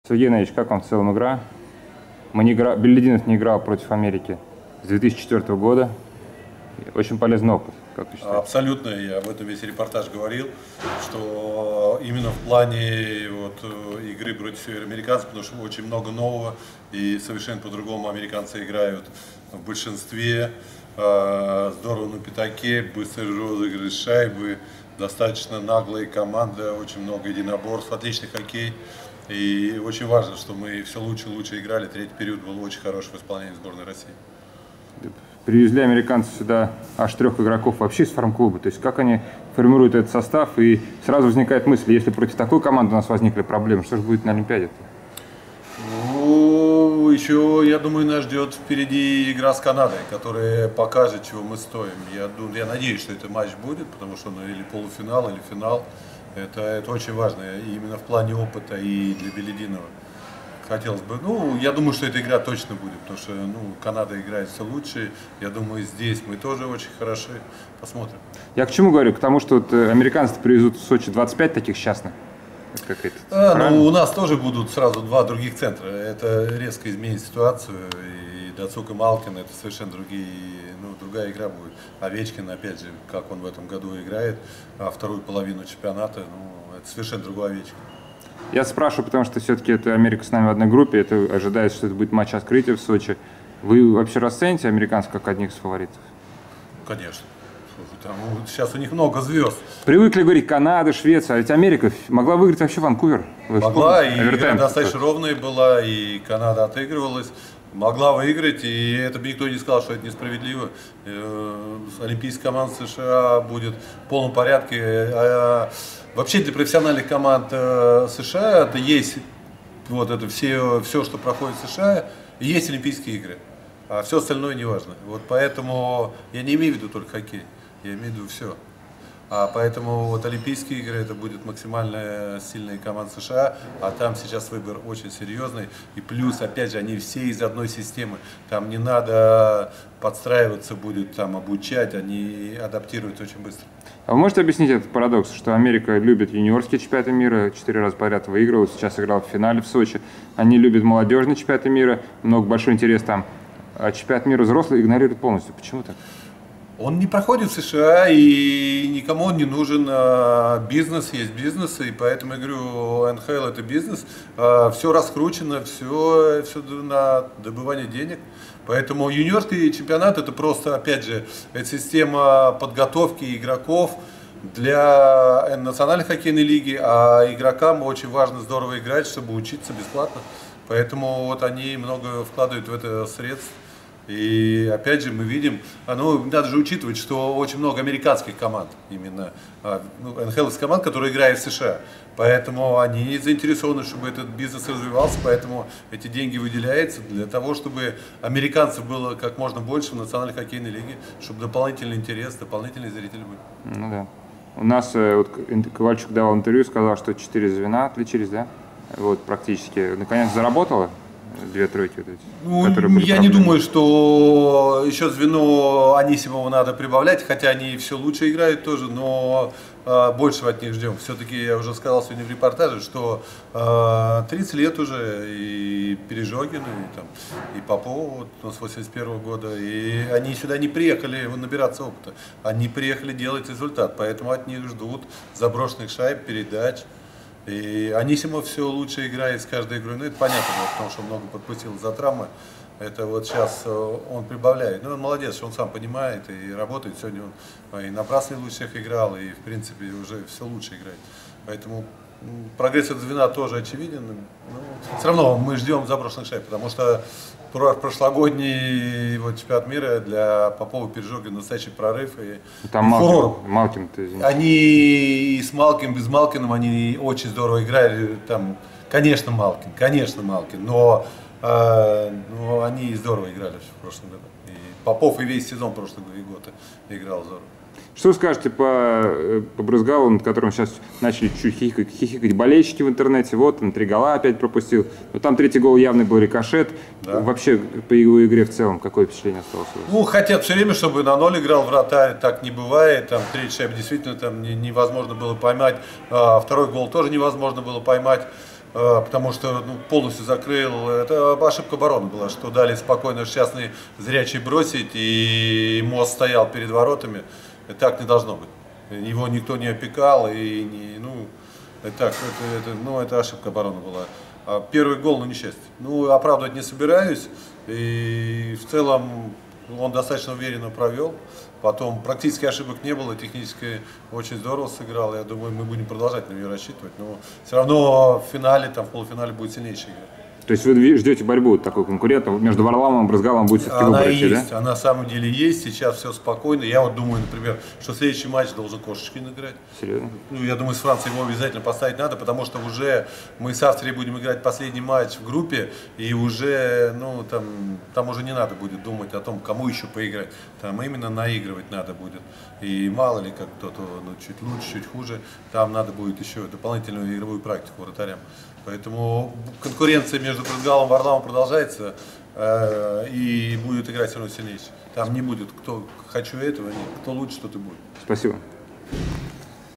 Сергей Гимаев - о матче с США (5:3) на ЧМ-2013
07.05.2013. Сергей Гимаев - о матче с США (5:3) на ЧМ-2013 в беседе с Павлом Лысенковым.